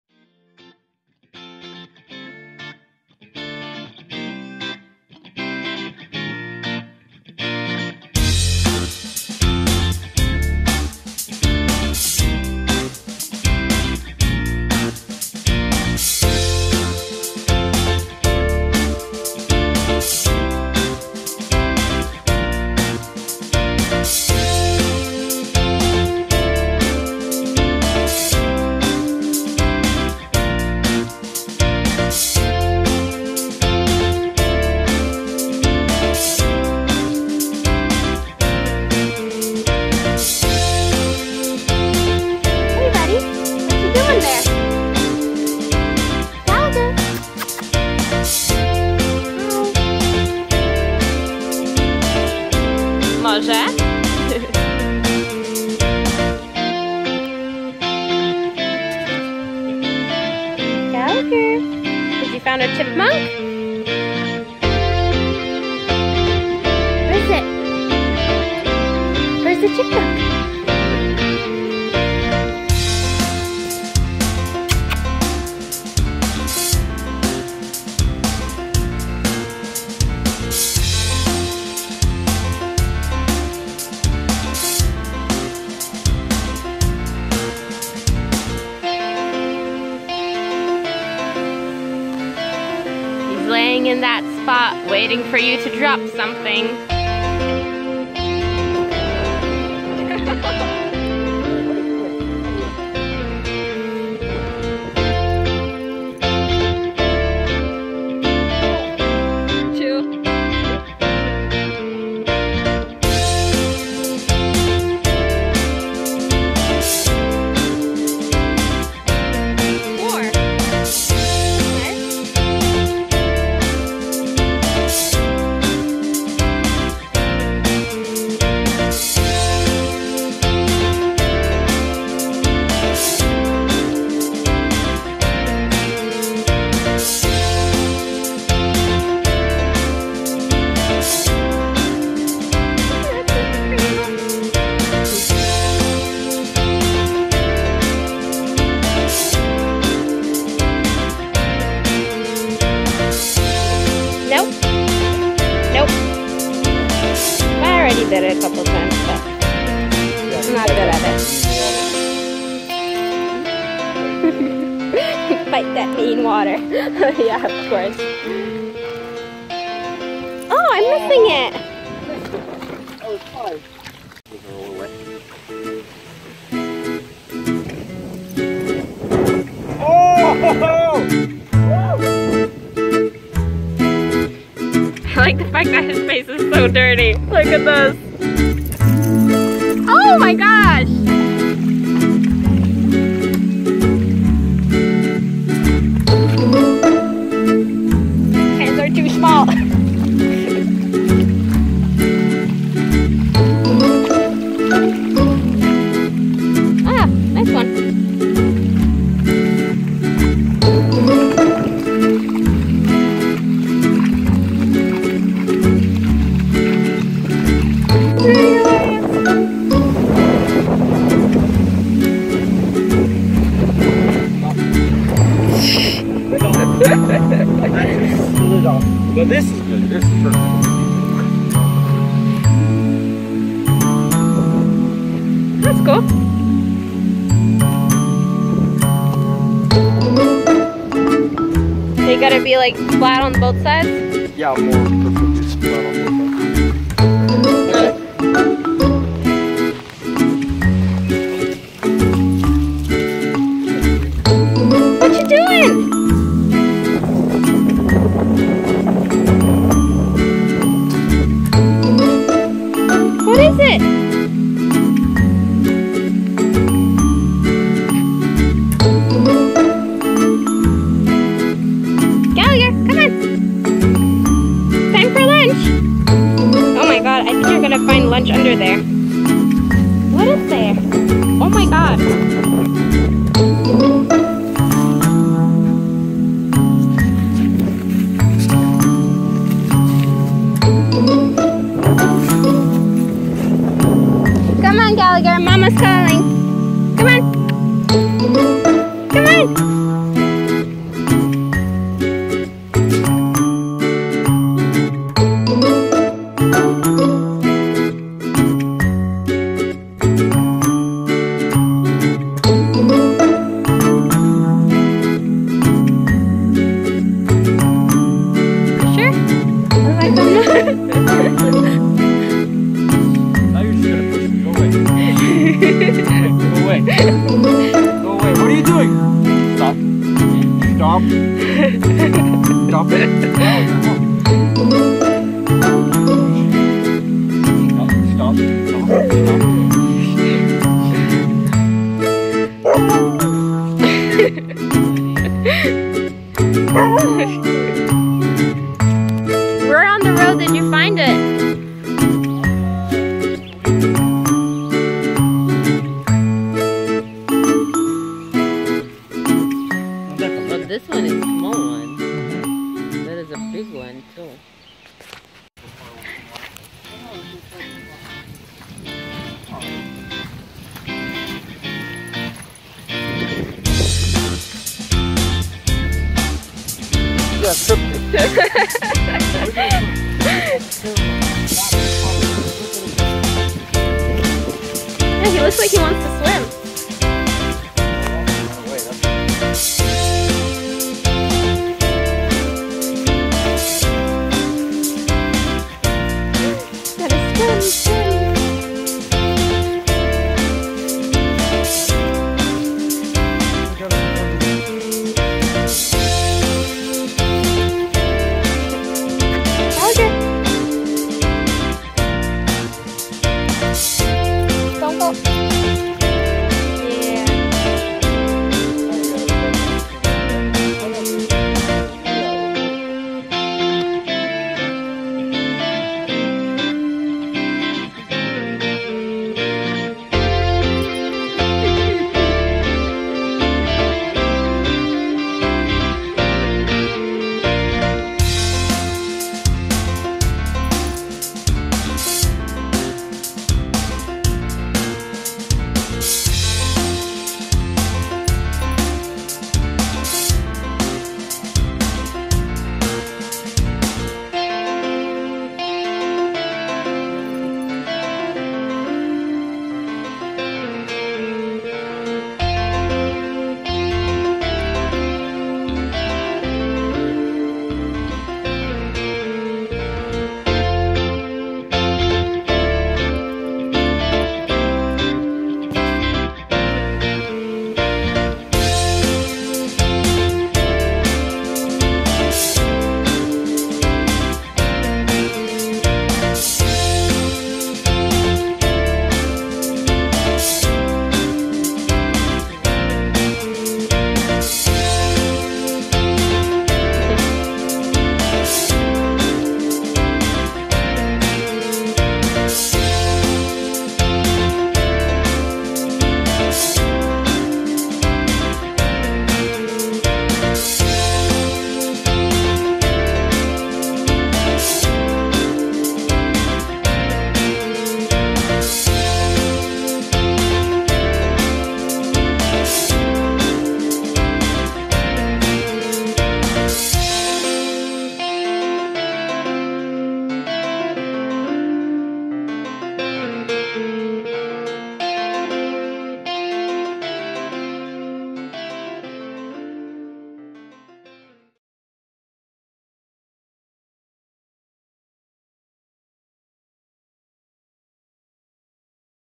First time Camping with Gally | Gally the Siberian Husky
Camping in Canada with Gally the adorable Siberian husky. Also, my friend found a turtle on the road so he brought it to the campground and we released it into the river.